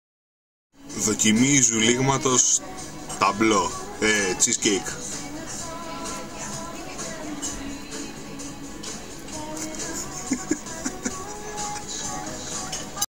Δοκιμή πλαστικοποίησης cheesecake
3-4 mm υποχώρηση επιφάνειας, μηδενικό λέρωμα χεριών: WIN!